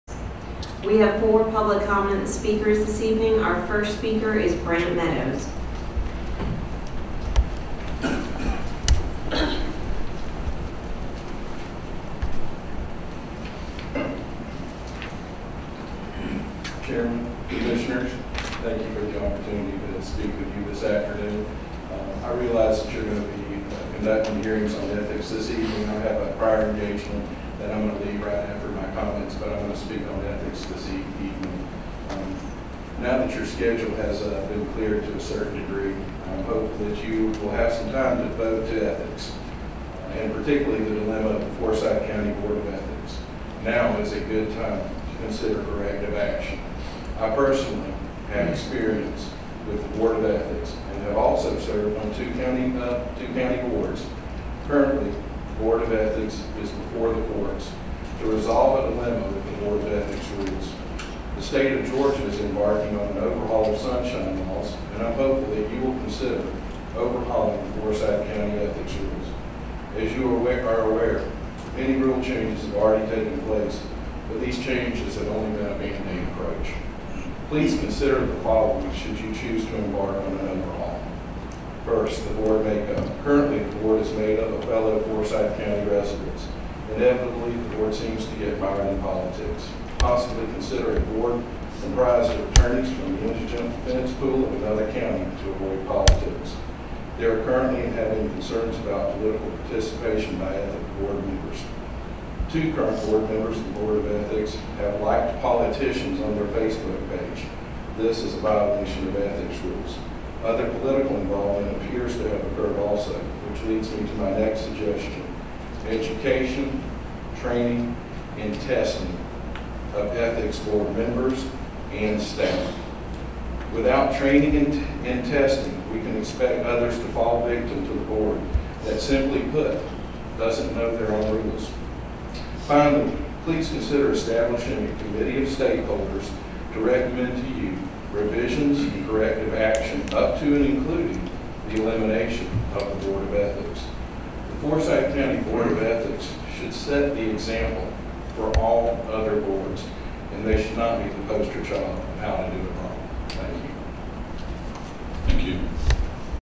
Brant Meadows — Forsyth County Ethics Board
Brant Meadows addresses Forsyth County Board of Commissioners on March 1, 2012.

Chairman, Commissioners, thank you for the opportunity to speak this afternoon.

Now that your schedule has been cleared, I am hopeful that you will have some time to devote to Ethics and particularly the dilemma of the Forsyth County Board of Ethics. Now is a good time to consider corrective action.

I personally have experience with the Board of Ethics and have also served on two Forsyth County boards.

Currently, the Board of Ethics is before the courts to resolve a dilemma with the board of ethics rules. The state of Georgia is embarking on an overhaul of sunshine laws and I am hopeful you will consider overhauling the Forsyth County ethics rules.

As you are aware, many rule changes have already taken place, but these changes have only been a band-aid approach.

Please consider the following, should you choose to embark on an overhaul.

1. Board make-up. Currently, the board is made up of fellow Forsyth County residents. Inevitably, the board seems to get mired in politics. Possibly, consider a board comprised of attorneys from the indigent defense pool of another county to avoid the politics.

2. There are currently and have been concerns about political participation by ethics board members. Two current members of Board of Ethics have liked politicians on their Facebook page. This is a violation of ethics rules. Other political involvement appears to have occurred also. Which leads me to the next suggestion.

3. Education, training and testing of ethics board members and staff. Without training and testing we can expect others to fall victim to a board that, simply put, doesn't know their own rules.

4. Finally, please consider establishing a committee of stakeholders to recommend to you revisions and corrective action up to and including the elimination of the Board of Ethics.

The Forsyth County Board of Ethics should set the example for all other boards to follow and not be the poster child of how to do it wrong.